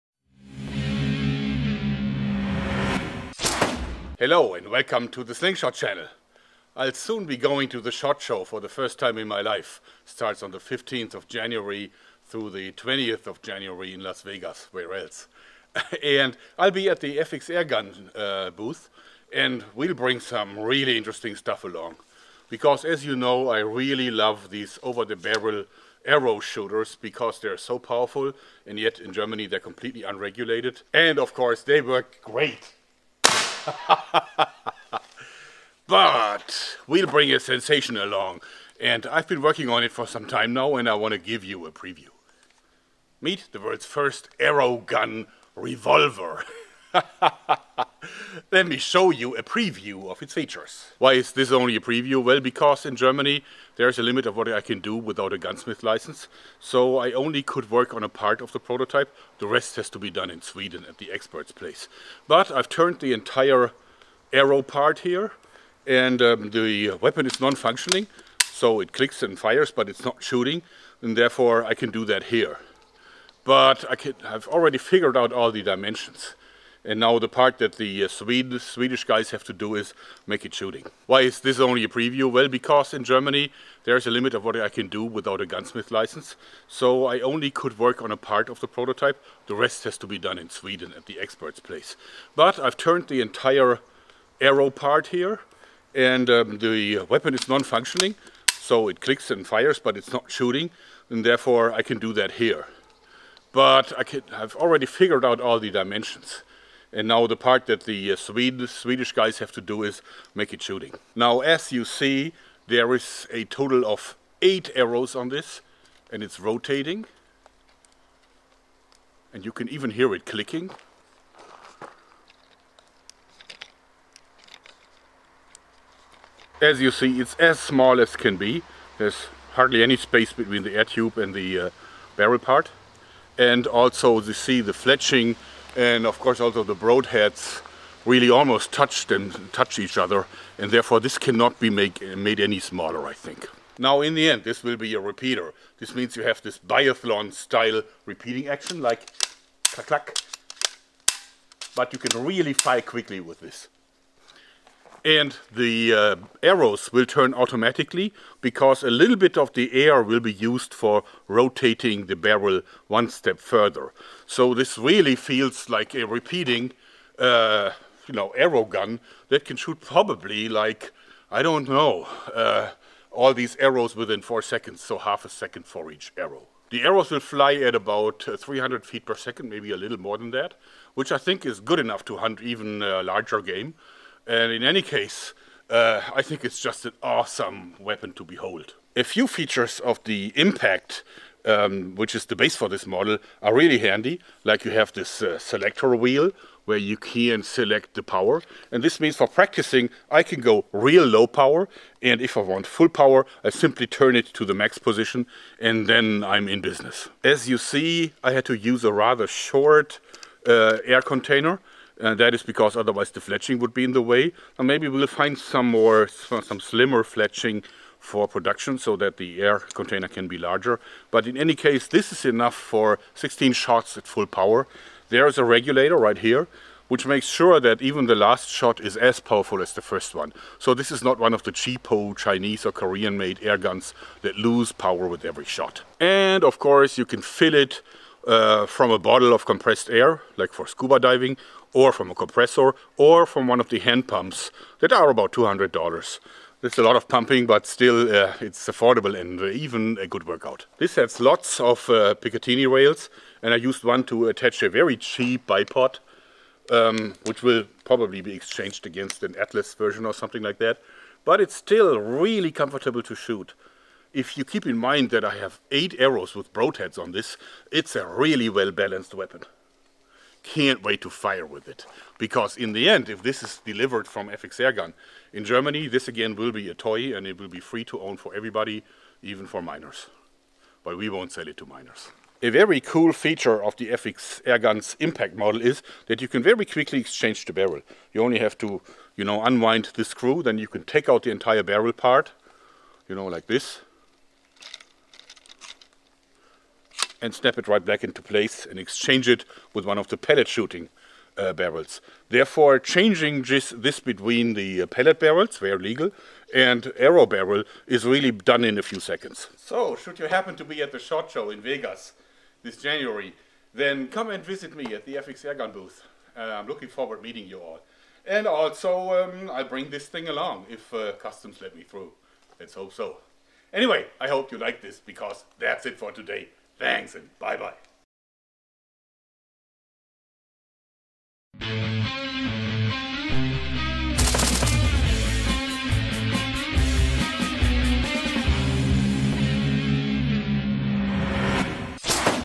Shot Show 2017 Sensation Unveiled!
Sorry for the double scene! Sloppy editing job, my bad.

Air powered arrow shooters are getting really popular because of their many advantages over conventional crossbows: High rate of fire, compact dimensions, high energy levels and amazing accuracy!

But so far they had one main issue in common with normal crossbows: One shot only, then you have to reload. This new product design study and prototype solves that problem by using a gatling-style 8 arrow drum! Now all you have to do is to cock back the valve hammer, "biathlon gun style", and then you can shoot again.

This amazing new invention will be on display at the 2017 Shot Show in Las Vegas booth 4049 (FXAirguns).